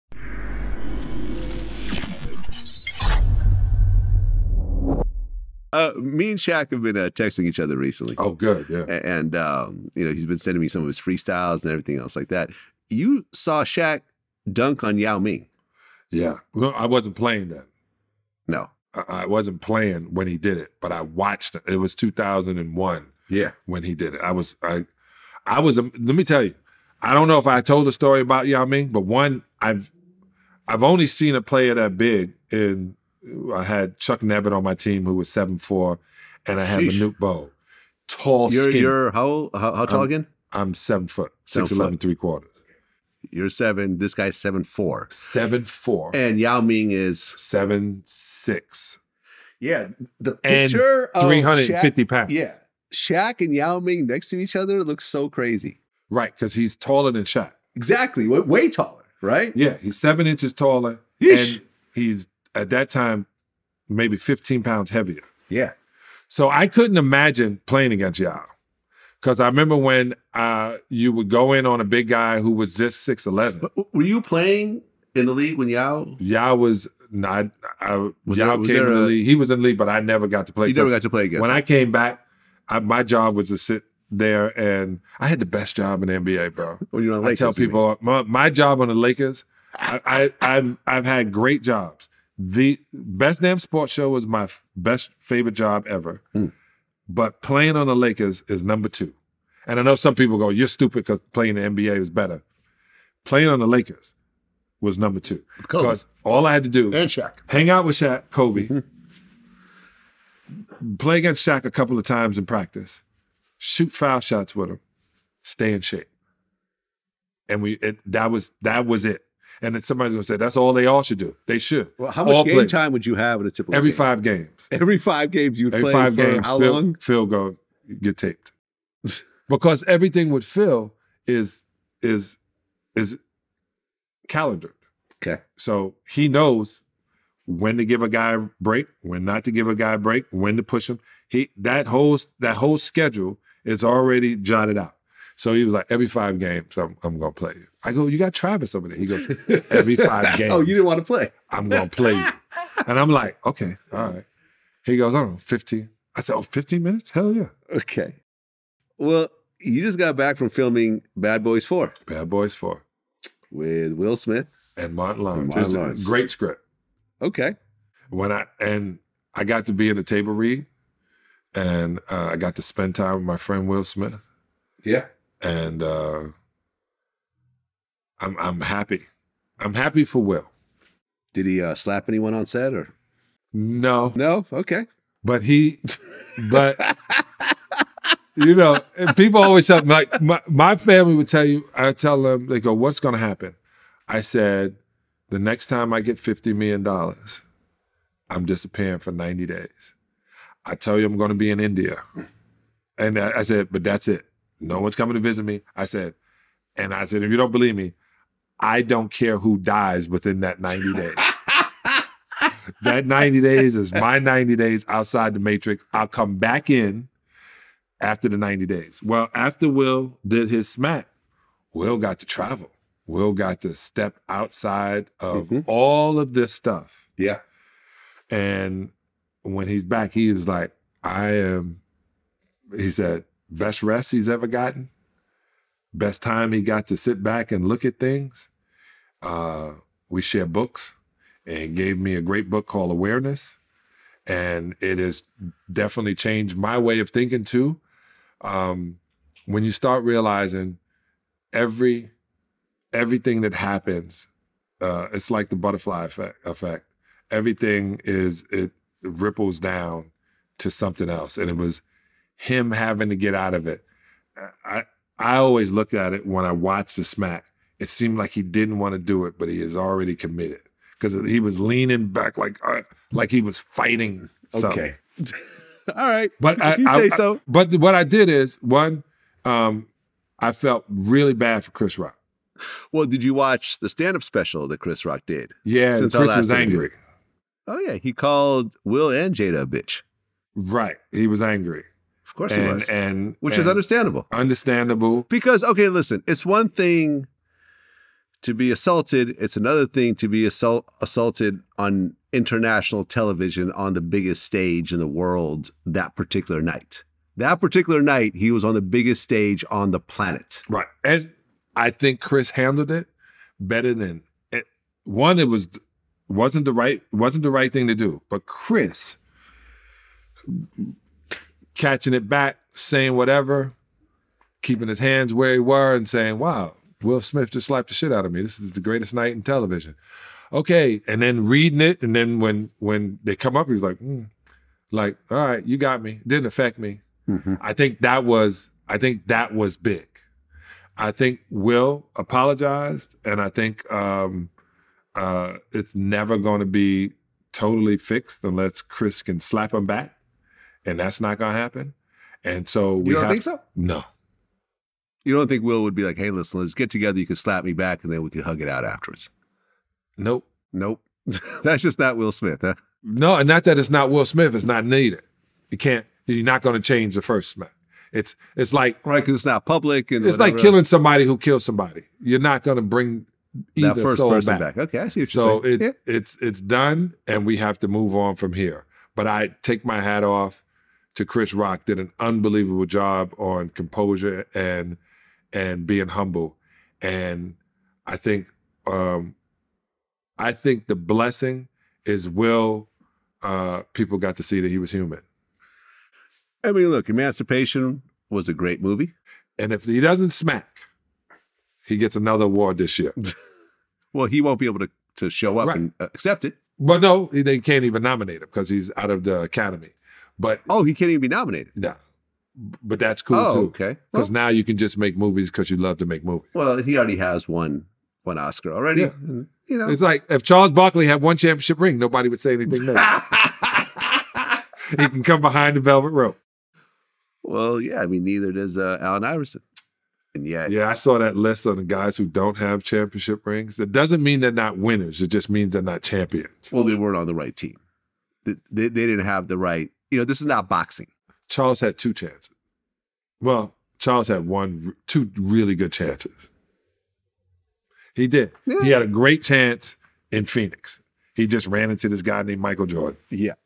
John Salley on Filming "Bad Boys 4", Will Smith Having "Best Time" Since Oscars Slap (Part 20)
In this clip, John Salley recounted being a member of the Lakers during the Shaq and Kobe era and watching Shaq dunk on Yao Ming. He described the sheer size of Yao who was 7’6 and well over 300 lbs and how he couldn’t fathom having to guard him. He also detailed why playing for that Lakers team was his second favorite job because he got to hang out with Shaq and Kobe, and he only played every 5 games.

Later, Vlad asked John Salley about filming scenes in “Bad Boys 4,” where he said he “got to spend time with my friend, Will Smith.” After saying that he’s “happy for Will,” Vlad jokingly asked if he slapped anyone, which led to John speaking about Will traveling the world after the Oscars incident. John stated that Will said it was the best rest he’s ever gotten, and he spoke about Will gifting him a book that changed the way he thinks. From there, John and Vlad discussed Chris Rock’s stand-up special, where he went at Will and Jada. John stated that Chris handled the slap better than most people, including not retaliating with violence.